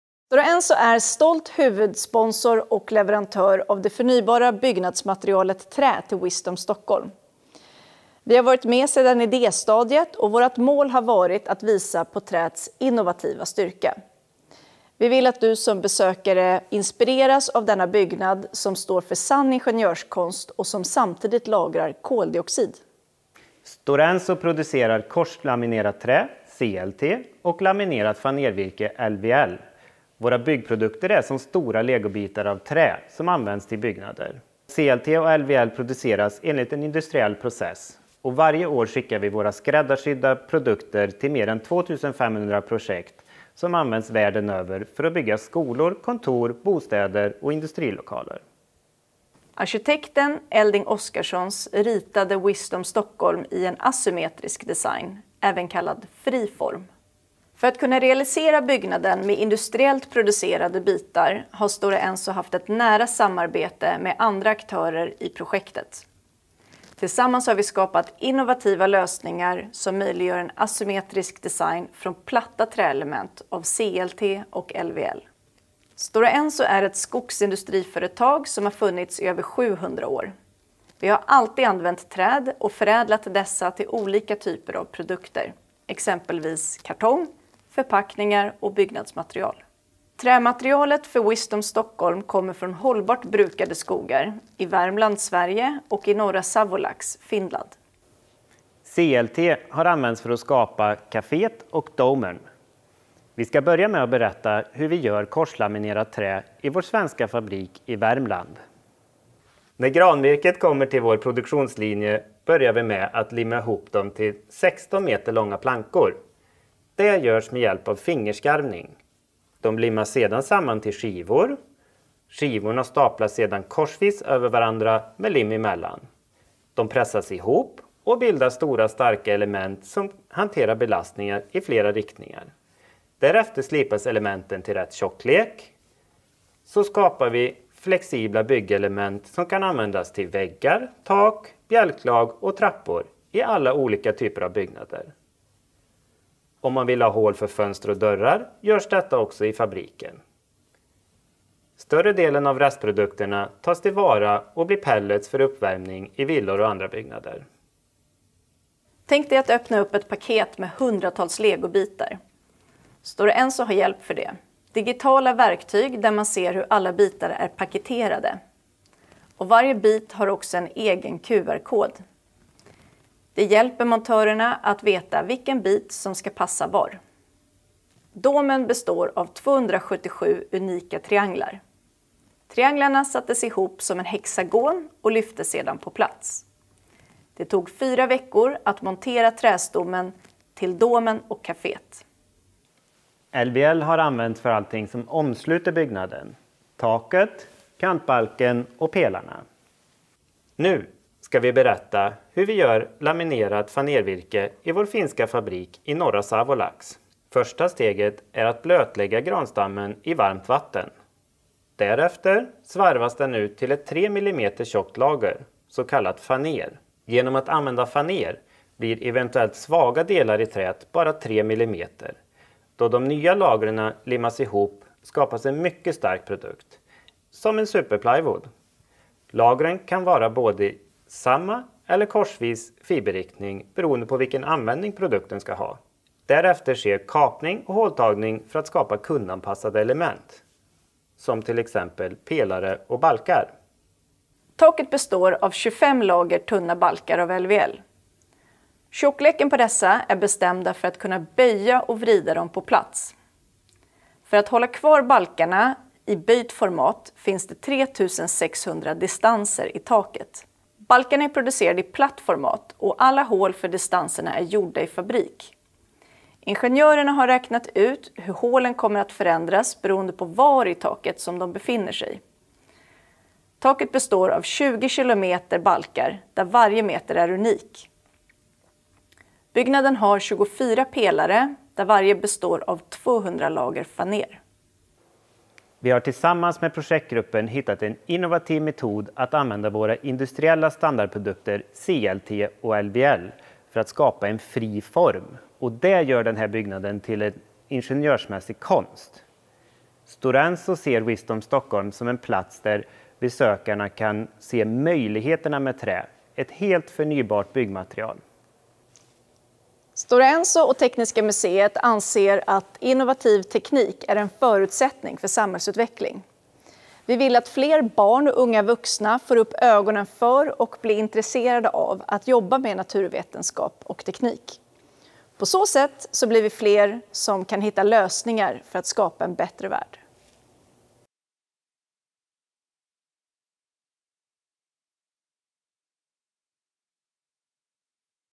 Wisdome Stockholm
Explore the awe-inspiring possibil-trees of wood construction. Watch this educational video with Jessika Szyber, Business Development Manager, Stora Enso and Carl Humble, Technical Project Manager, Stora Enso. The video is in Swedish with English subtitles.